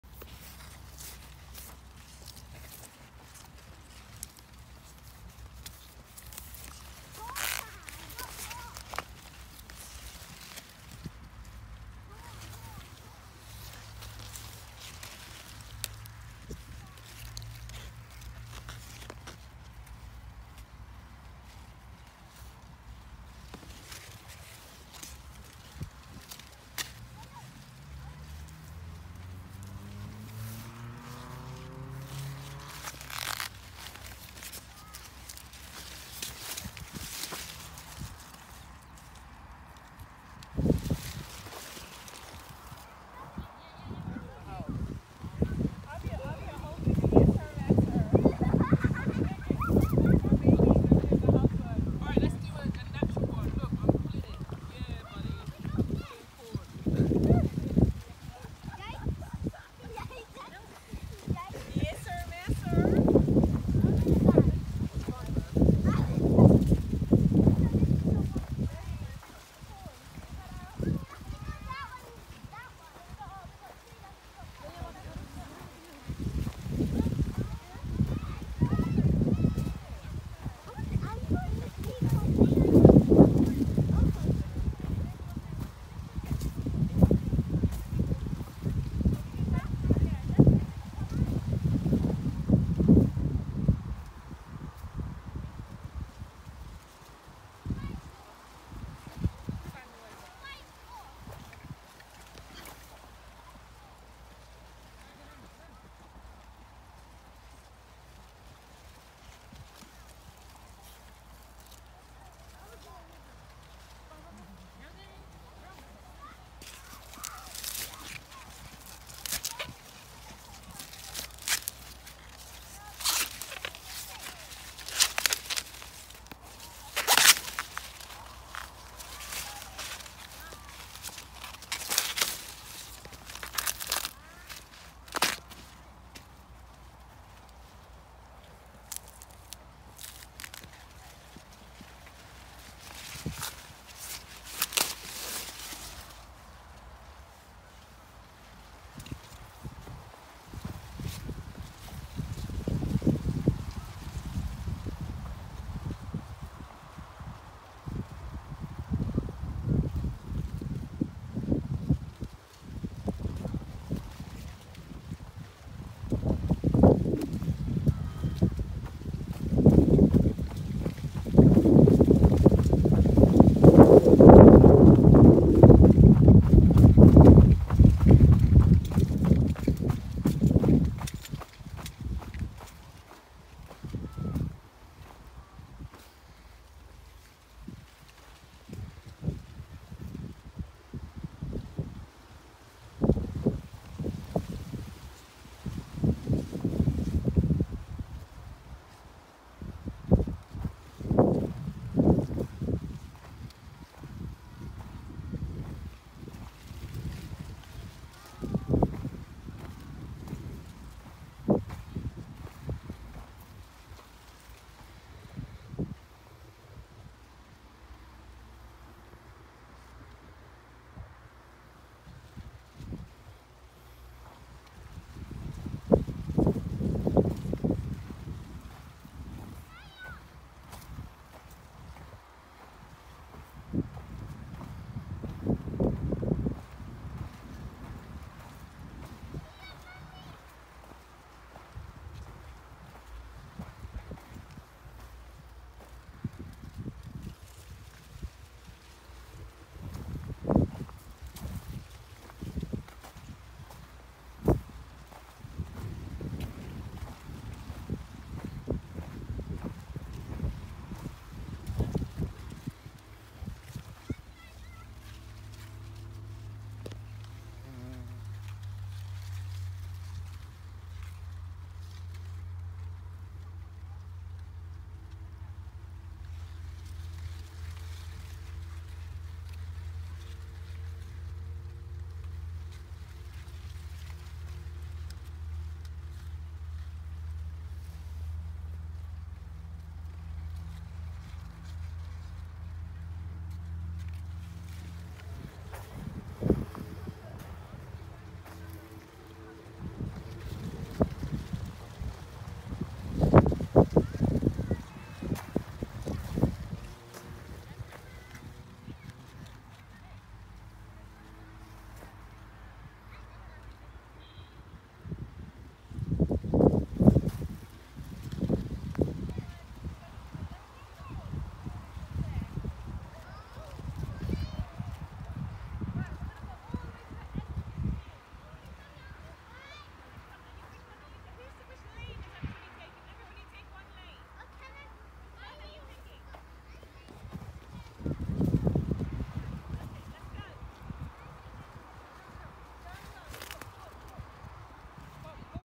Can anyone tell me what is growing in Corn ? - Sweet Corn Farm in the UK - sweet corn Farm in Kent.
Can anyone tell me what extra growing in Corn ?